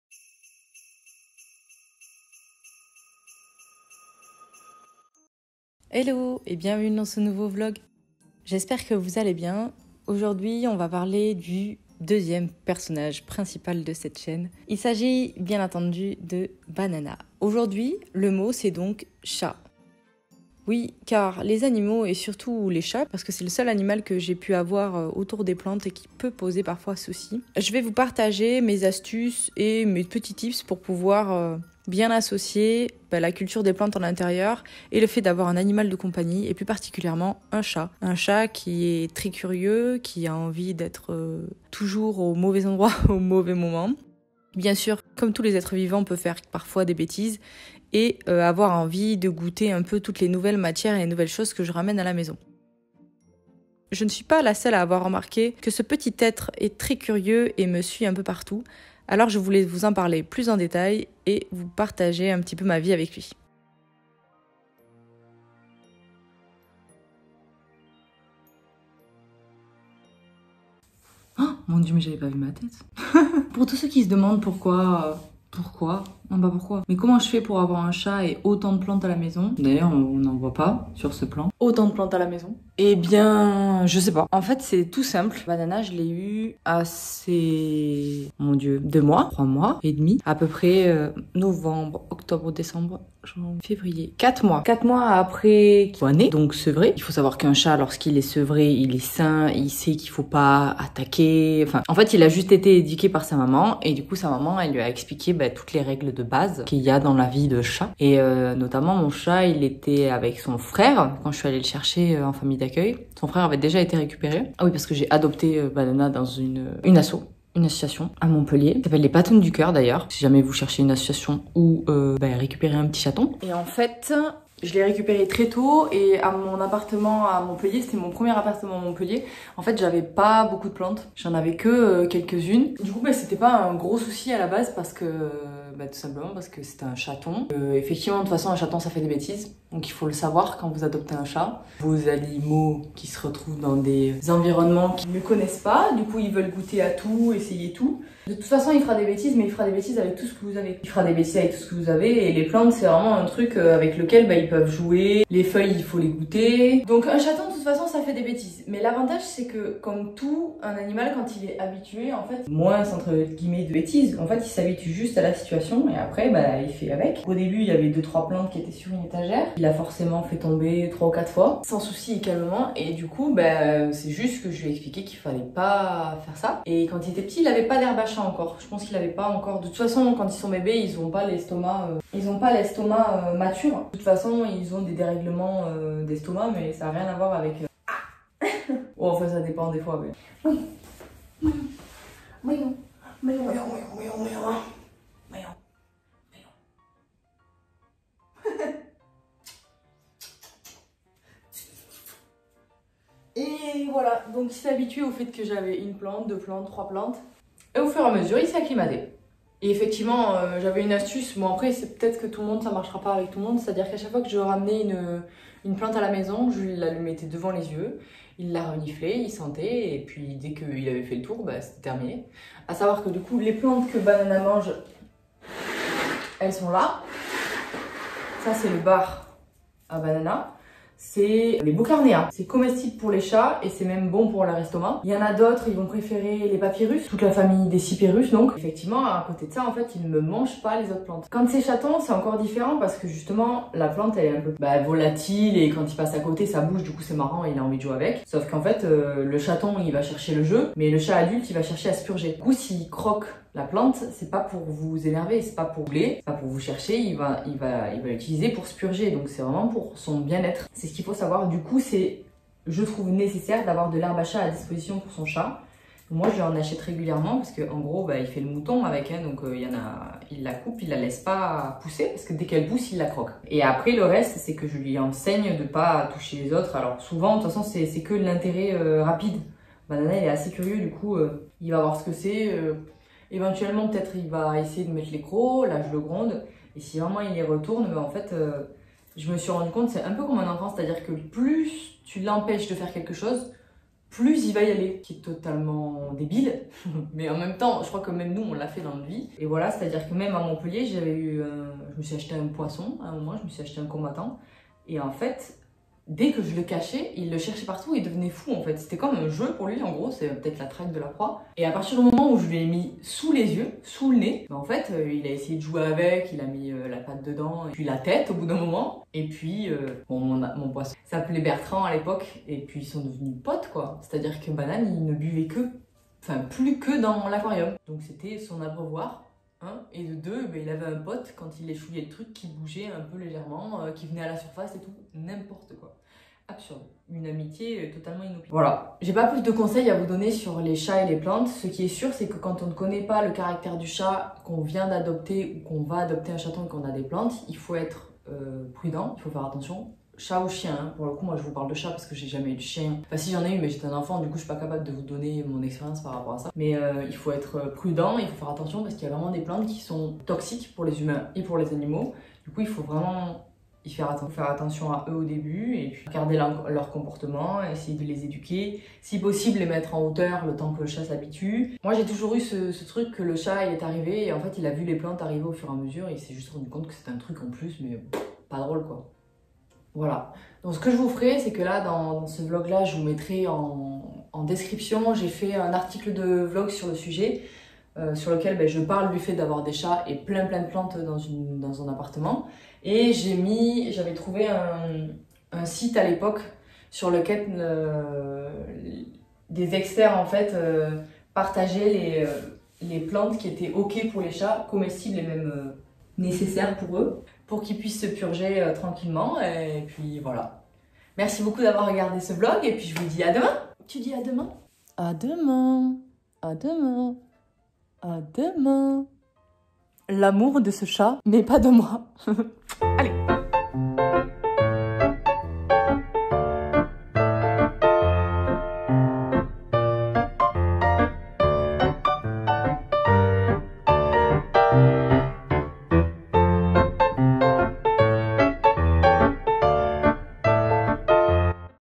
Avoir un chat et une centaine de plantes ! Vlogmas | Vertbobo
Hello !

Une petite vidéo de ma vie avec mon chat et toutes mes plantes !
Et le site avec toutes les infos des plantes non toxiques pour les chats